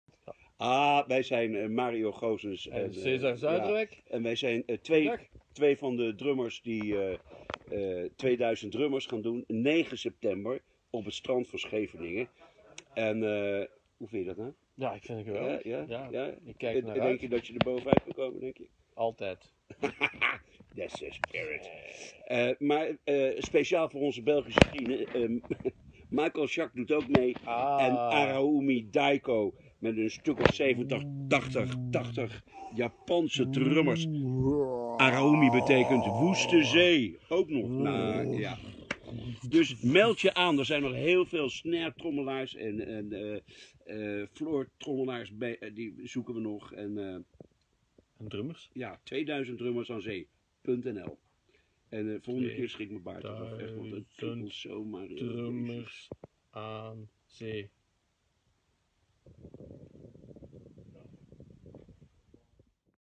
Mario Goossens (Triggerfinger) en Cesar Zuiderwijk
Mario en Cesar over het 2000 drummers aan zee project op 9 september voor meer info ga naar en schrijf je in